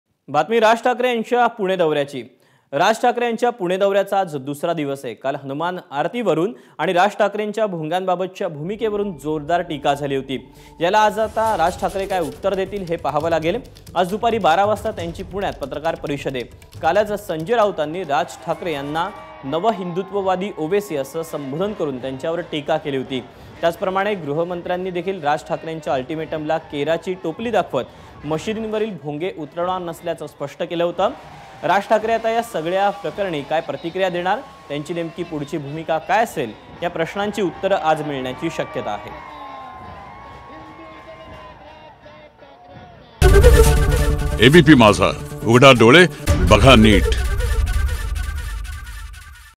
Raj Thackeray: हुनमान जयंतीवरुन 'राज'कारण, राज ठाकरे टीकाकारांना उत्तर देणार ? ABP Majha
हुनमान जयंतीवरुन 'राज'कारण, राज ठाकरे टीकाकारांना उत्तर देणार ?